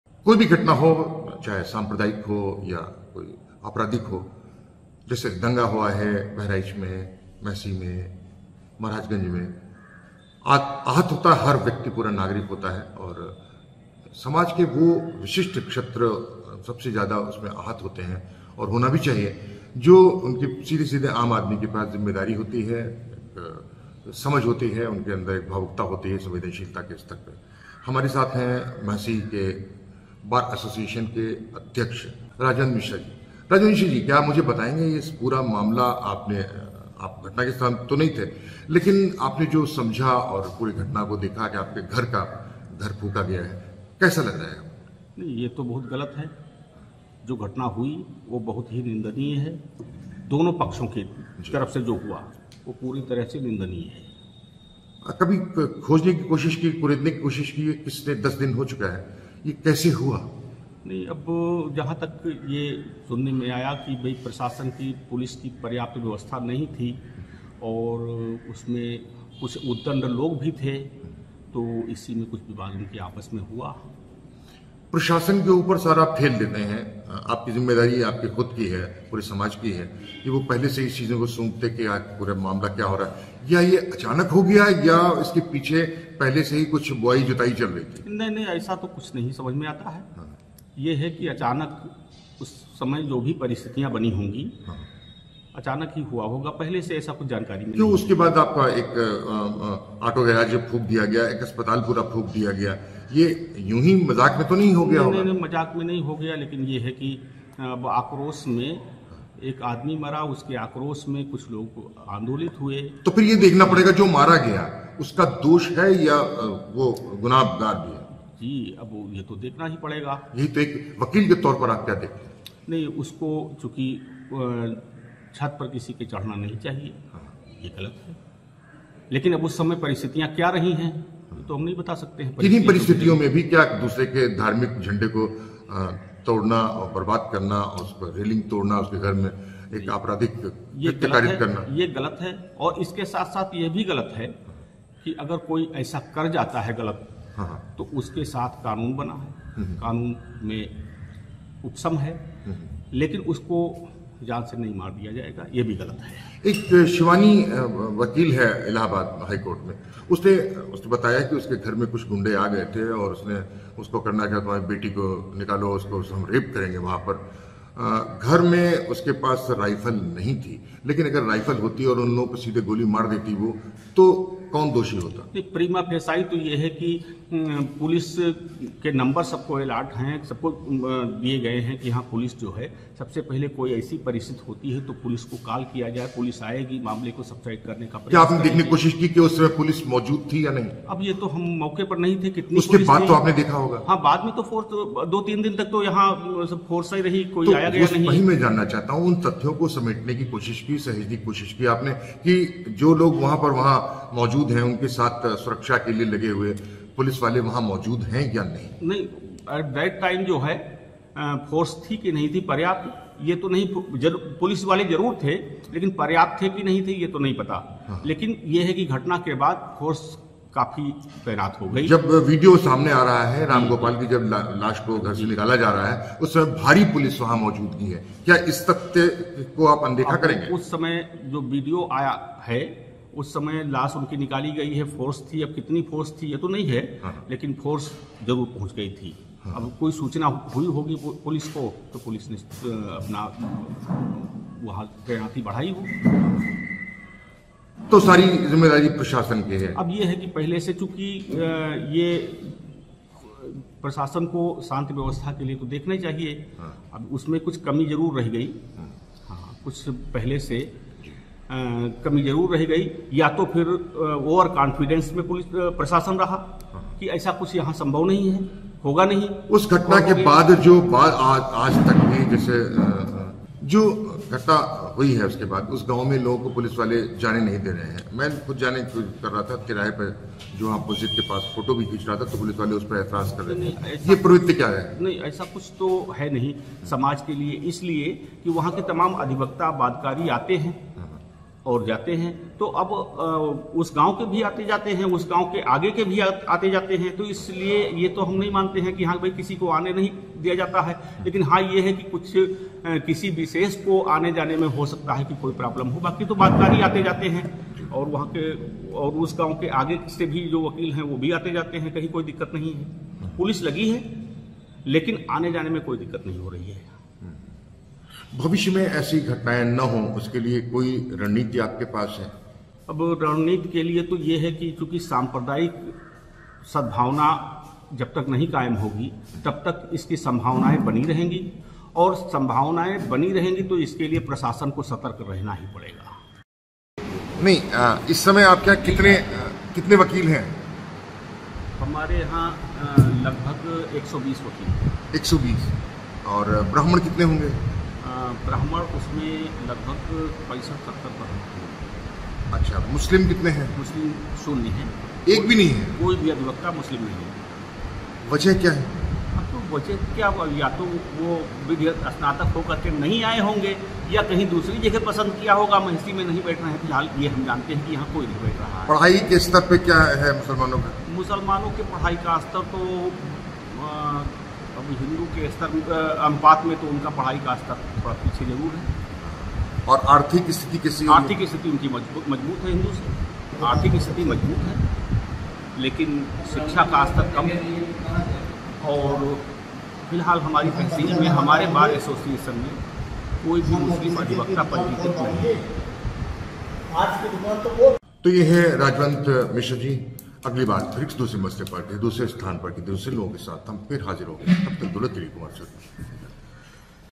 घर में घुस कर धार्मिक झंडा फाडना गलत, लेकिन पुलिस को खबर तो देते
बहराइच महसी तहसील बार अध्यक्ष रजवंत मिश्र से बातचीत
मौके पर मौजूद पुलिस को हस्तक्षेप करना चाहिए था, जांच जरूरी
मुसलमानों की आर्थिक दशा बेहतर, शिक्षा में फिसडडी
दोलत्ती की खबरों को शेयर कर घंटी भी दें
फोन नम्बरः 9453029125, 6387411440